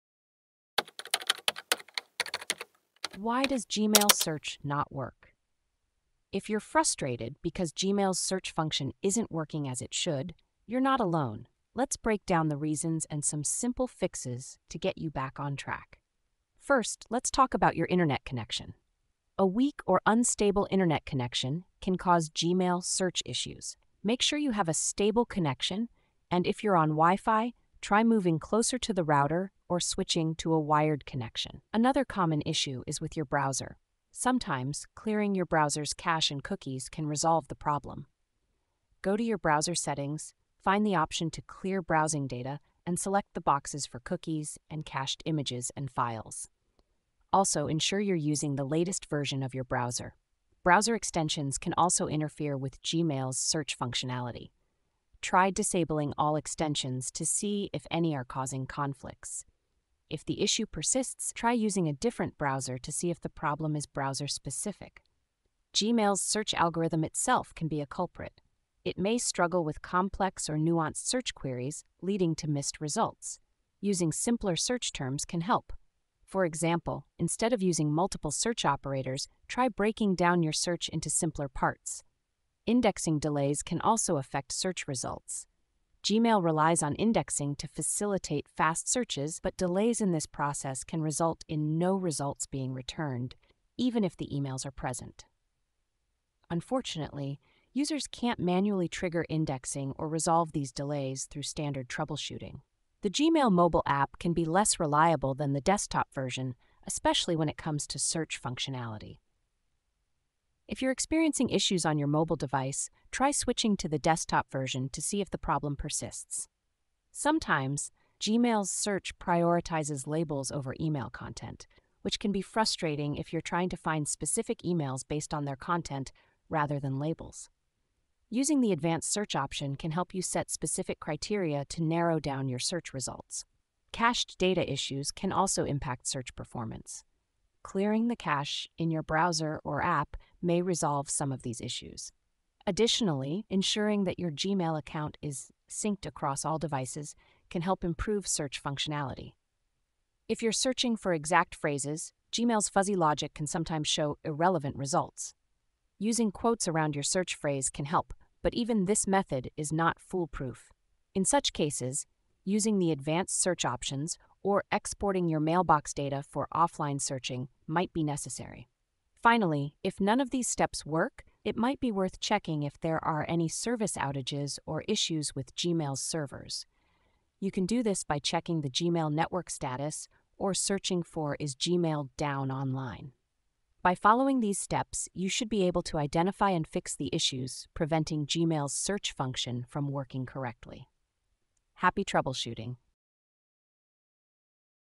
Why Does Gmail Search Not Work? - TheEmailToolbox.com
Why Does Gmail Search Not Work? If you've been encountering issues with Gmail's search functionality, you're not alone. In this informative video, we’ll cover some of the common reasons why you might be having trouble and provide you with simple solutions to get things running smoothly again. We will discuss how your internet connection, browser settings, and even the Gmail app itself can impact your search experience.

Additionally, we’ll highlight the importance of using the right search terms and techniques, as well as how to manage cached data effectively. We’ll also touch on how Gmail prioritizes labels over email content and what you can do to refine your searches for better results. If you’ve tried everything and still can’t find what you’re looking for, we’ll guide you on how to check for potential service outages.

Join us as we troubleshoot these common issues and help you regain control over your Gmail search.